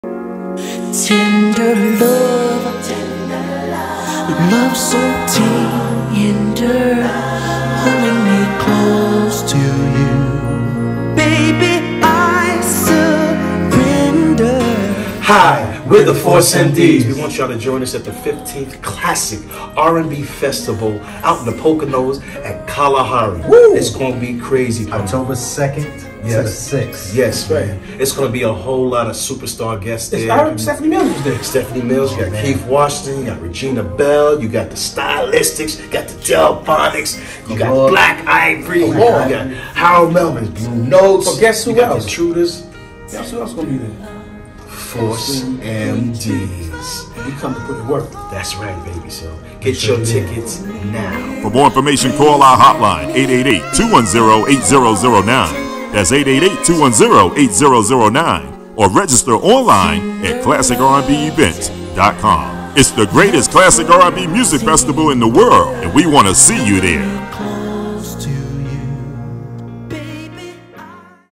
THE FORCE MDs invites you to the 15th Classic R&B Music Festival Oct. 2-6, 2022
THE FORCE MDs invites you to the 15th Classic R&B Music Festival Oct.